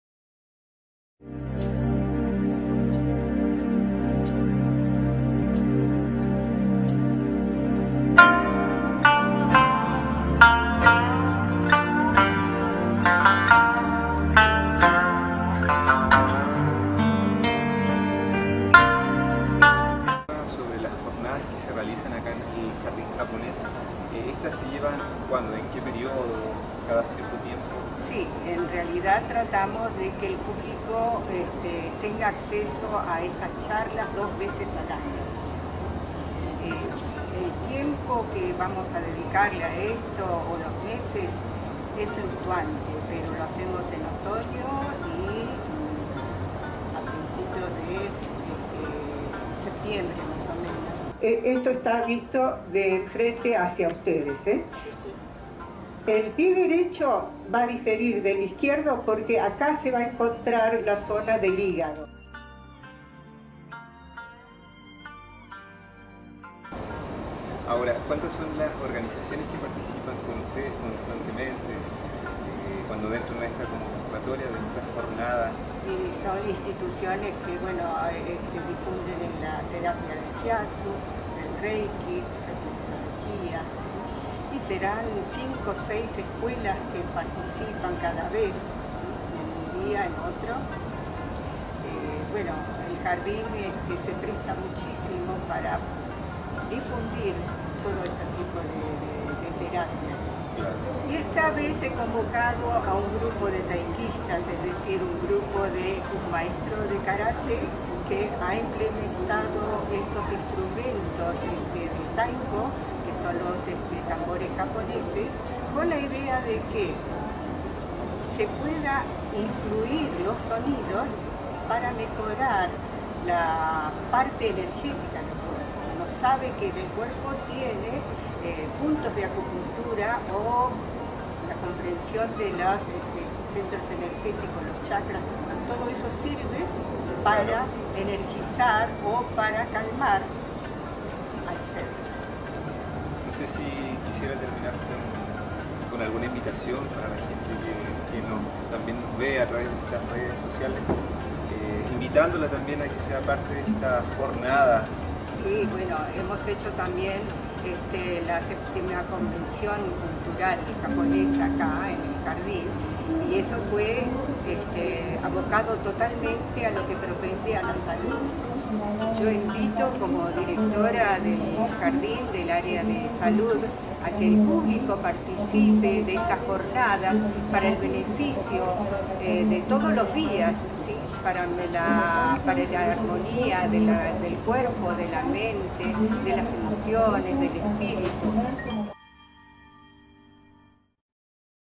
Jornadas de Salud en el Jardín Japonés [Entrevista]
Nuestro corresponsal de Chile, Mario Guardia-Hino, entrevistó a Michiko Arakaki, quien ofreció una charla y taller de reflexología en el Jardín Japonés durante las Jornadas de Salud del mes de noviembre.

Periodista y cámara: Mario Guardia-Hino
Edición: Mario Guardia-Hino
Sonido: Nahuel Murru
Revisión: M.Laura Martelli